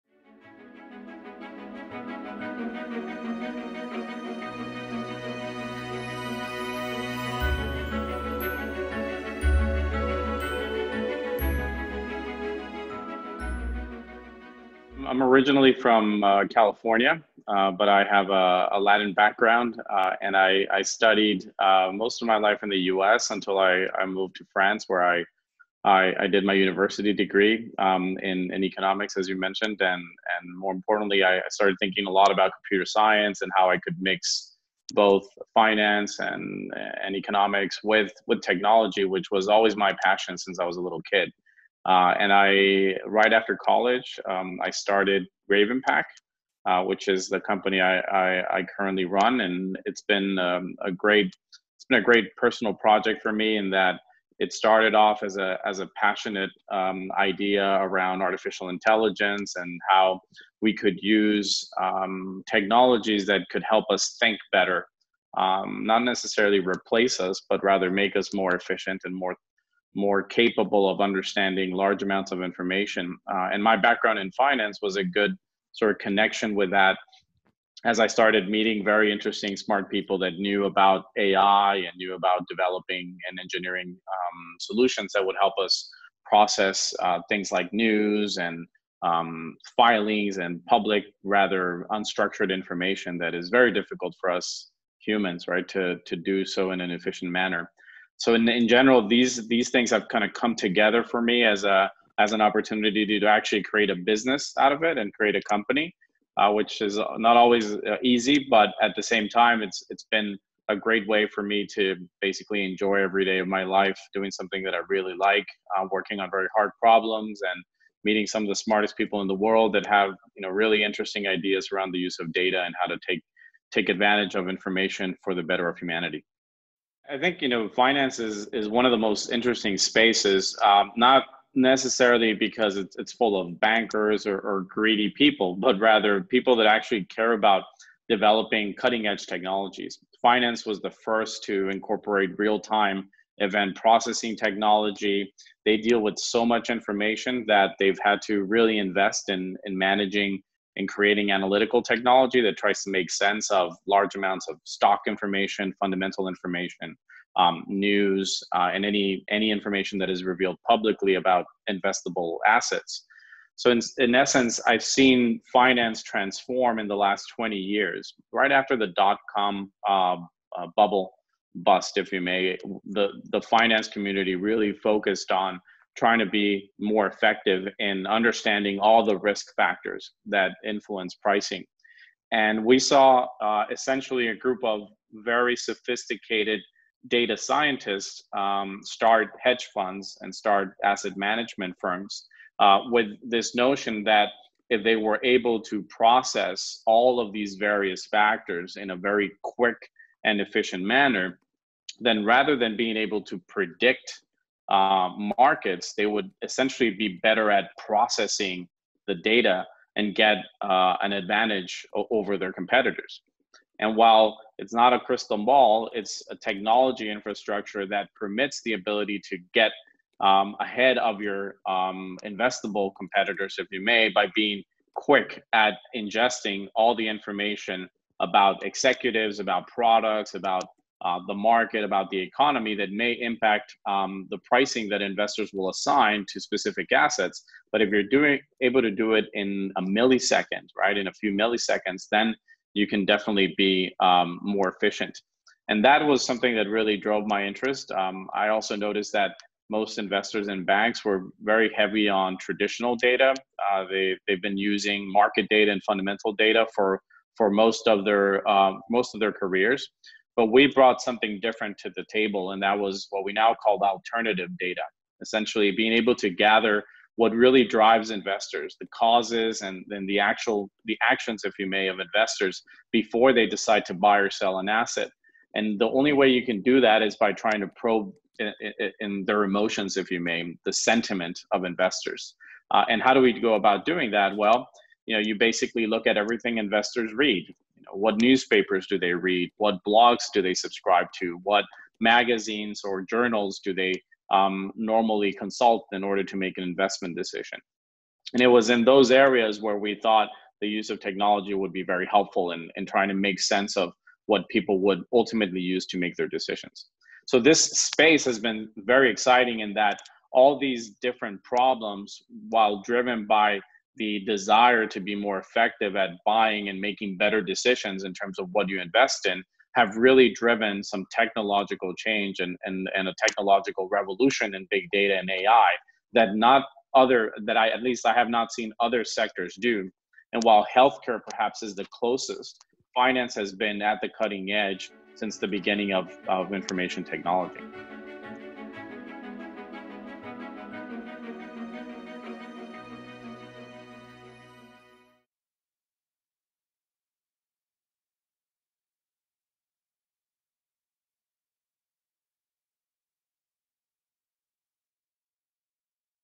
Profiles: Armando Gonzalez, Technology Entrepreneur, CEO and Co-Founder of RavenPack
Armando Gonzalez is a technology entrepreneur and the CEO and Co-founder of RavenPack, the leading provider of bigdata analytics for financial institutions. At RavenPack, he oversees all product design and engineering of the company’s data products and analytical tools. Armando is a recognized expert in applied big data and artificial intelligence (AI) technologies. He is widely regarded as one of the most knowledgeable authorities on systematic data analysis in finance.

Armando Gonzalez is a recognized speaker at academic and business conferences across the globe. Armando holds degrees in Economics and International Business Administration from the American University in Paris. As a thought leader, his commentary and research has appeared in leading business publications such as the Wall Street Journal, Financial Times, among many others.